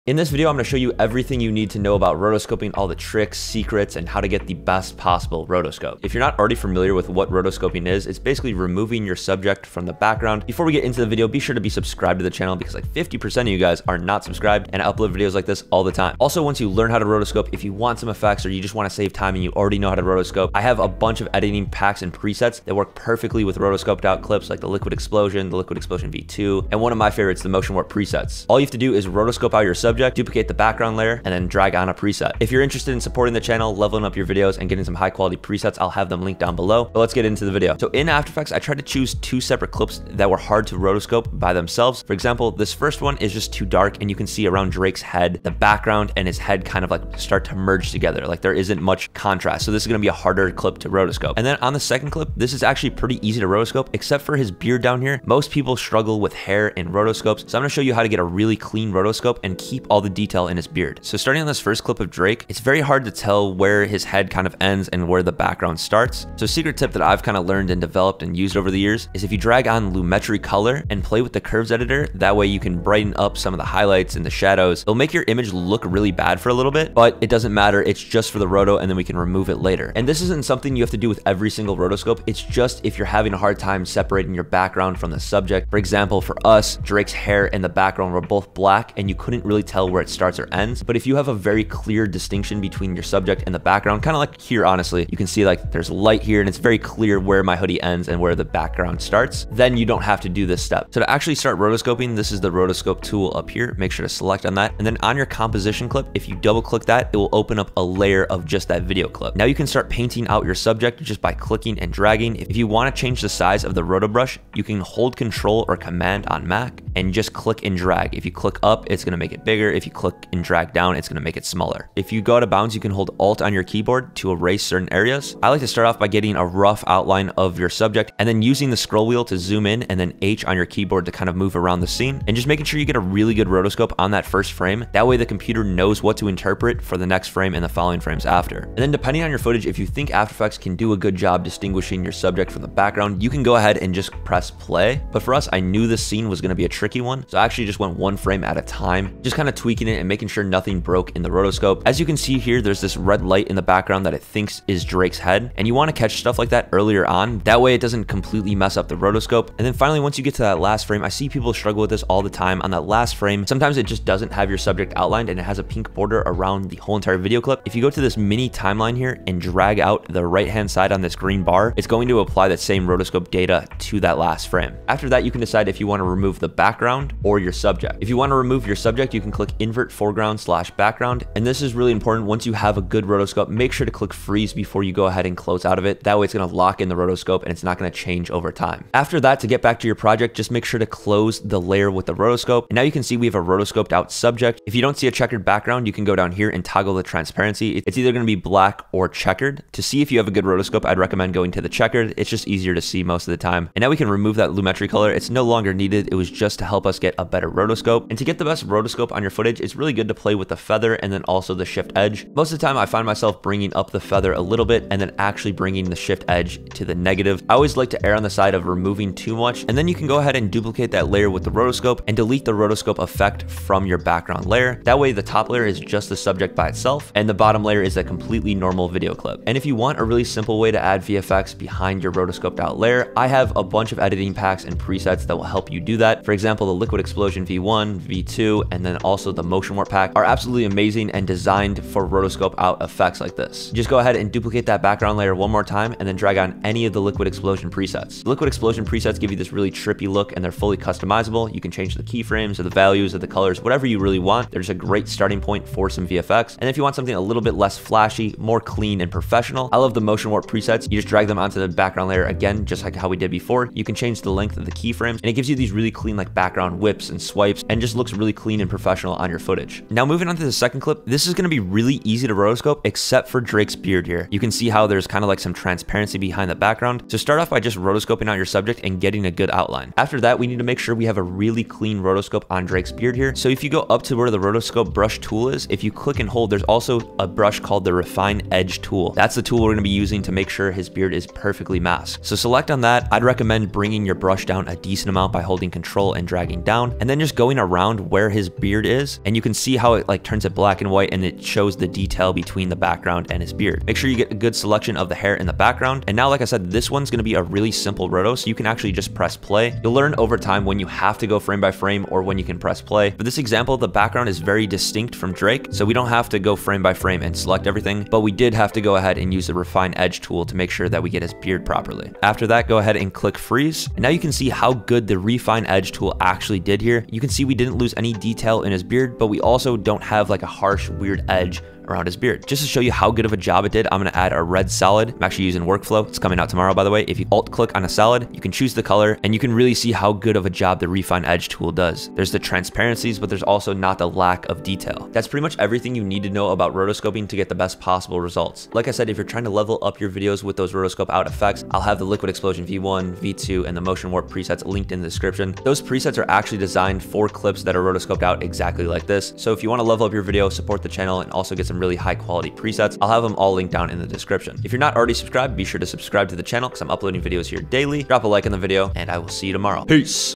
ROTOSCOPE SECRETS that NOBODY wants you to know... (How to Rotoscope in After Effects)
In this adobe after effects tutorial we go over how to rotoscope in after effects. Rotoscoping is great to add effects to your music video as effects or transitions. How to rotoscope, rotoscope tricks, rotoscope after effects 2023, rotoscope 2.0